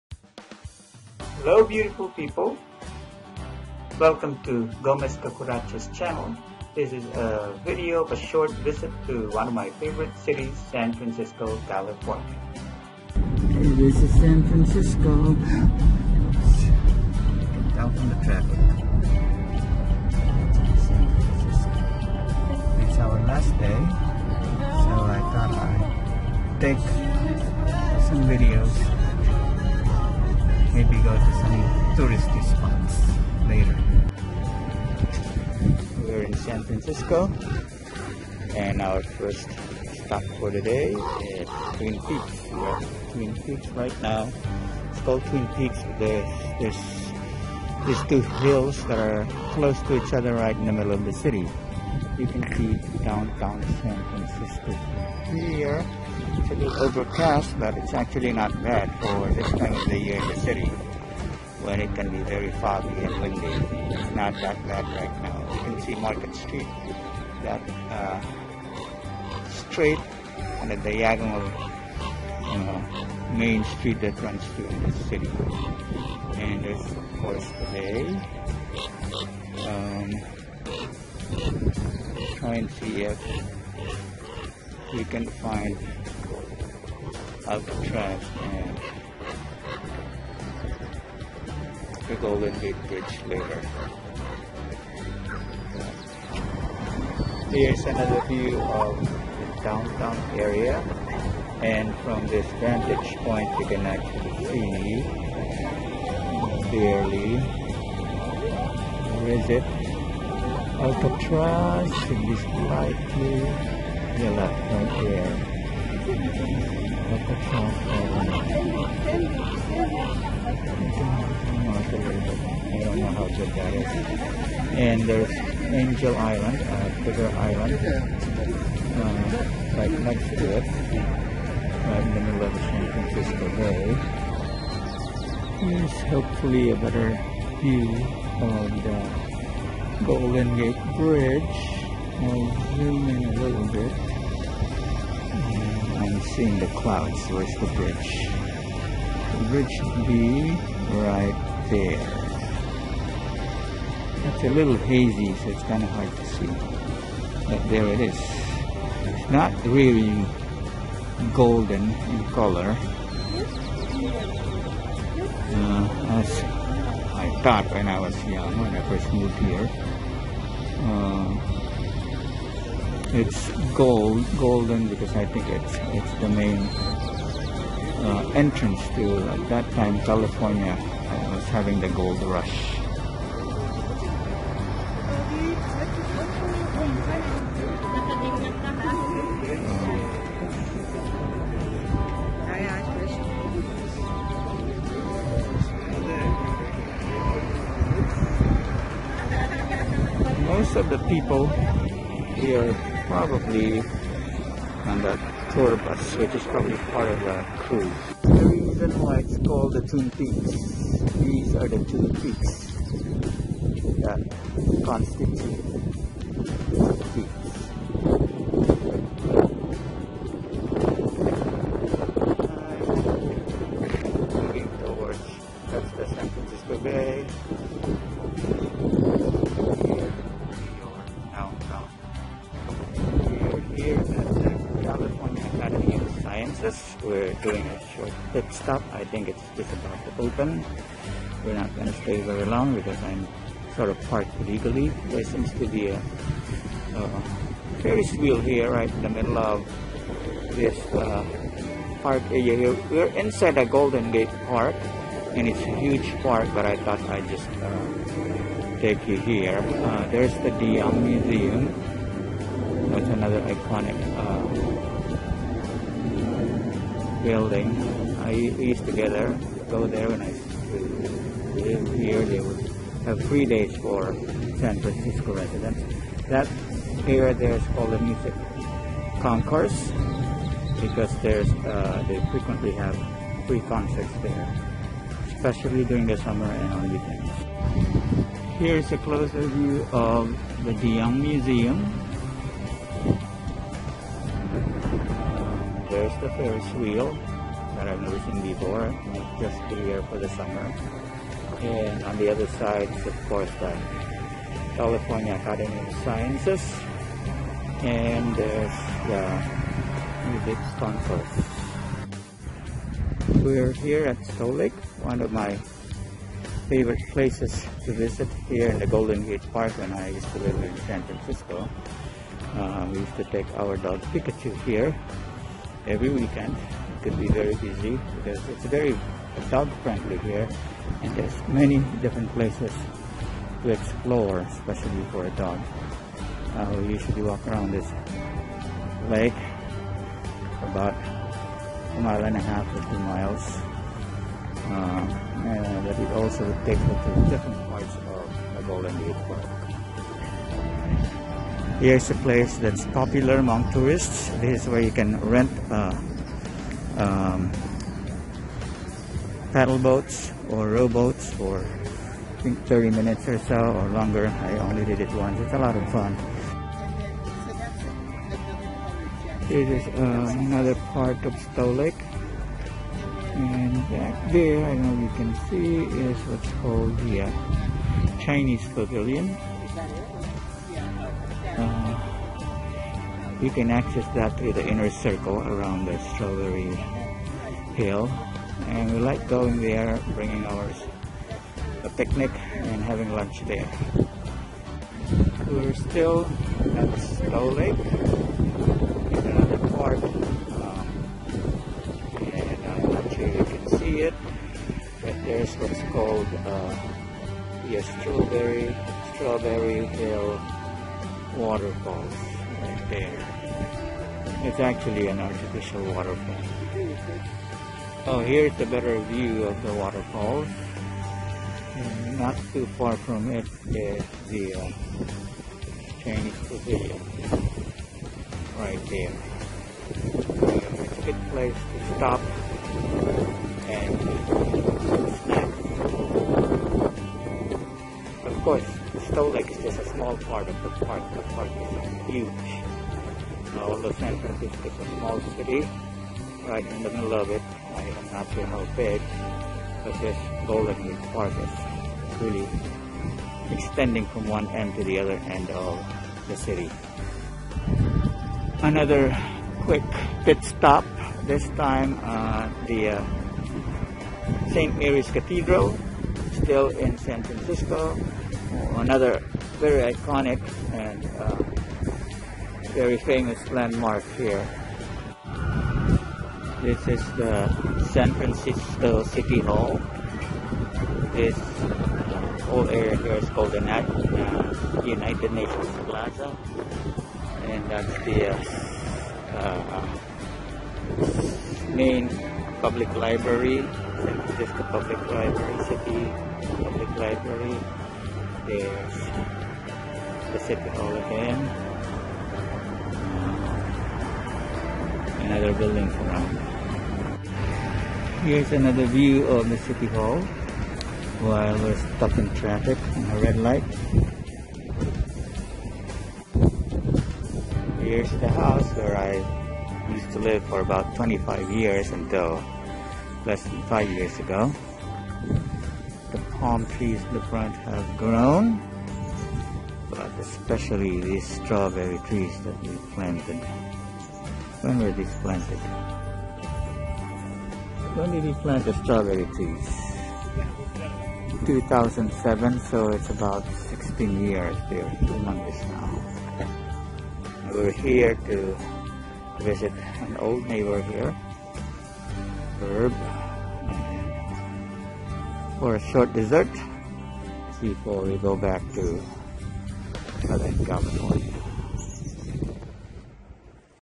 California Walking Tour: Short San Francisco Visit
Join us as we visit what used to be my home for 25 years. This quick tour of San Francisco includes visits to Twin Peaks, Goldengate Parke, California Academy of Science, De Young Museum, Music Concourse, Stow Lake, Saint Mary's Cathedral, and SF City Hall.